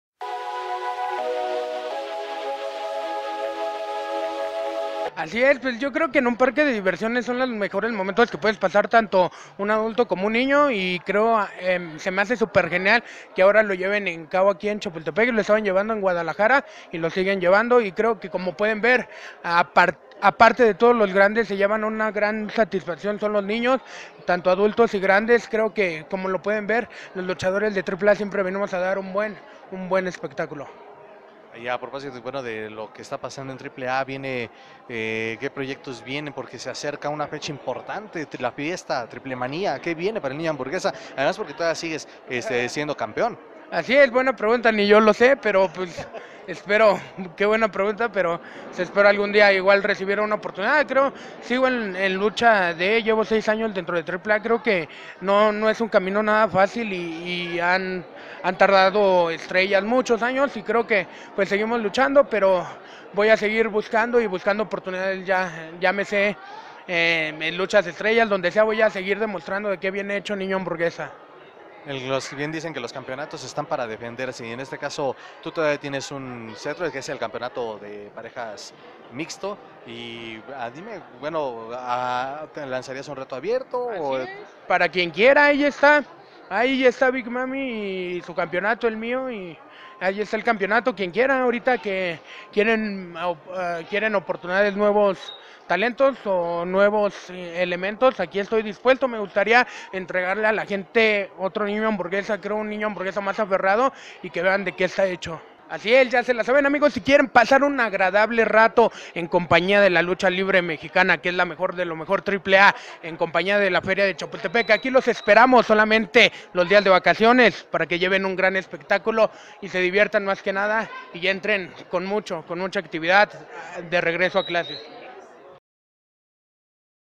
Niño Hamburguesa desde La Feria de Chapultepec
Niño Hamburguesa destacó la iniciativa de Lucha Libre Triple A con La Feria de Chapultepec para el presente periodo vacacional